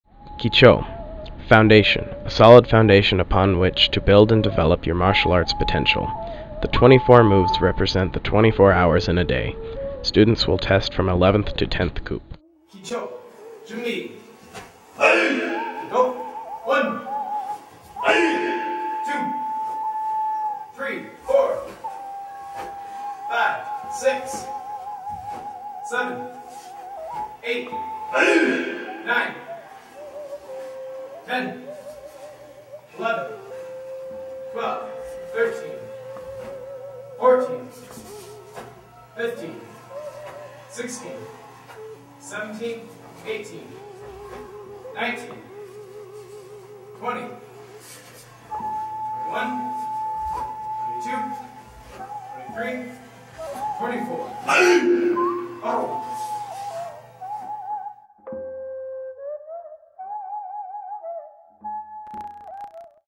Kicho
Foundation: A solid foundation upon which to build and develop your martial arts potential.

Basic Ready Stance
1.Left leg steps forward into Left Front Stance: Left Down Block, Kihap!
2.Left Middle Block.
3.Right Front Snap Kick into
4.Right Front Stance: Right Middle Punch.
5.Left Front Snap Kick into
6.Left Front Stance: Left Middle Punch
7.Right leg steps forward into a Right Front Stance: Right Middle Punch.
8.Left Middle Punch, Kihap!
9.Left leg turns 270° CCW toward C into Left Back Stance: Left Inside Forearm Block.
10.Left leg shifts into Left Front Stance facing C: Left Down Block.
11.Left Middle Block.
12.Right Front Snap Kick into
13.Right Front Stance: Right Middle Punch.
14.Right leg turns 180° CW toward D into Right Back Stance: Right Inside Forearm Block.
15.Right leg shifts into Right Front Stance facing D: Right Down Block.
16.Right Middle Block.
17.Left Front Snap Kick into
18.Left Front Stance: Left Middle Punch.
19.Left leg turns 90° CCW towards A into a Left Front Stance: Left Down Block.
20.Right foot steps up to left, left steps forward into Left Front Stance: Left Middle Block.
21.Right leg steps forward into Right Front Stance: Right Middle Punch.
22.Left leg steps forward into a Left Front Stance: Left Middle Punch.
23.Right leg steps forward into Right Front Stance: Right Middle Punch.
24.Left Middle Punch, Kihap!
Left Foot Returns

Kibon Joonbi
1.Left leg steps forward into Wen Ahp Sogi: Wen Hadan Maki, Kihap!
2.Wen Chungdan Maki.
3.Orun Ahp Chagi into
4.Orun Ahp Sogi: Orun Chundan Joomok Chirigi.
5.Wen Ahp Chagi into
6.Wen Ahp Sogi: Wen Chundan Joomok Chirigi.
7.Right leg steps forward into Orun Ahp Sogi: Orun Chundan Joomok Chirigi.
8.Wen Chundan Joomok Chirigi, Kihap!
9.Left leg turns 270° CCW toward C into Wen Dwi Sogi: Wen Ahn Palmok Maki.
10.Left leg shifts into Wen Ahp Sogi facing C: Wen Hadan Maki.
11.Wen Chundan Maki.
12.Orun Ahp Chagi into
13.Orun Ahp Sogi: Orun Chundan Joomok Chirigi.
14.Right leg turns 180° CW toward D into Orun Dwi Sogi: Orun Ahn Palmok Maki.
15.Right leg shifts into Orun Ahp Sogi facing D: Orun Hadan Maki.
16.Orun Chundan Maki.
17.Wen Ahp Chagi into
18.Wen Ahp Sogi: Wen Chundan Joomok Chirigi.
19.Left leg turns 90° CCW towards A into Wen Ahp Sogi: Wen Hadan Maki.
20.Right foot steps up to left, left steps forward into Wen Ahp Sogi: Wen Chundan Maki.
21.Right leg steps forward into Orun Ahp Sogi: Orun Chundan Joomok Chirigi.
22.Left leg steps forward into Wen Ahp Sogi: Wen Chundan Joomok Chirigi.
23.Right leg steps forward into Orun Ahp Sogi: Orun Chundan Joomok Chirigi.
24.Wen Chundan Joomok Chirigi, Kihap!
Wen Bal Paro